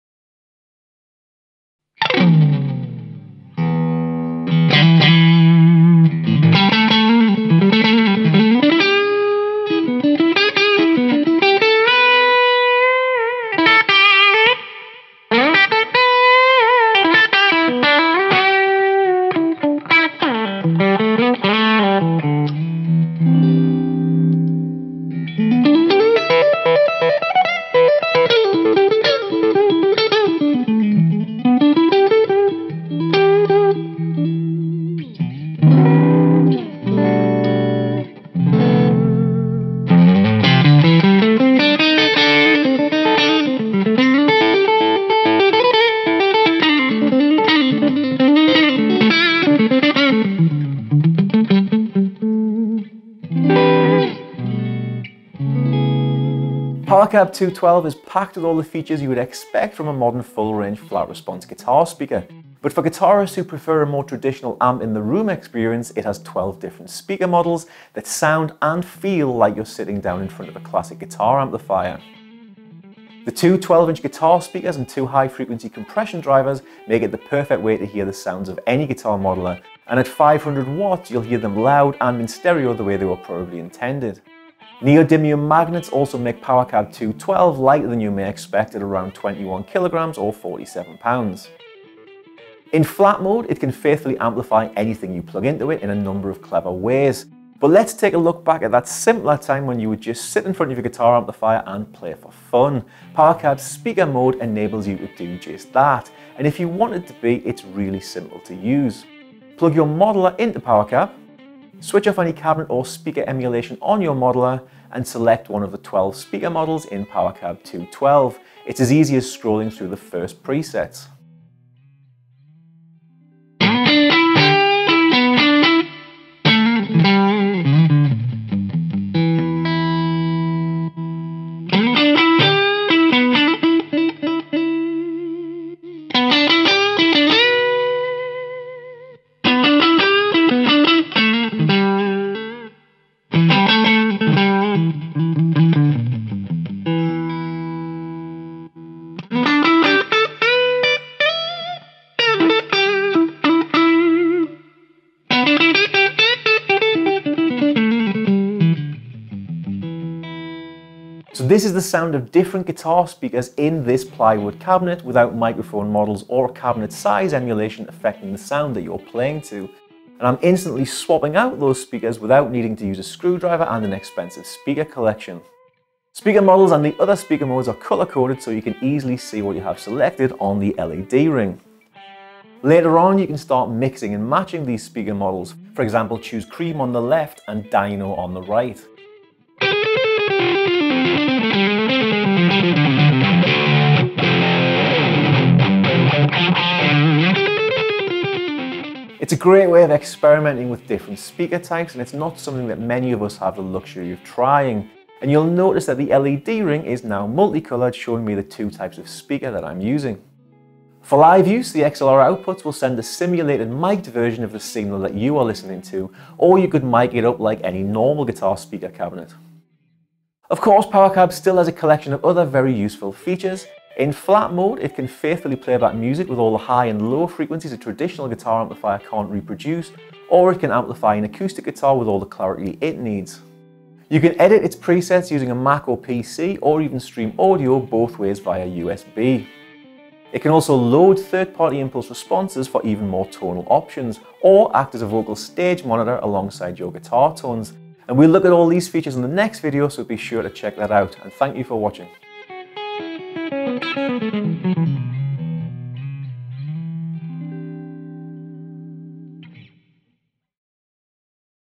Powercab 212 Plus Overview | Line 6
Join Paul Hindmarsh on a tour of the Powercab 212 Plus, the premier amplifier for your modeler.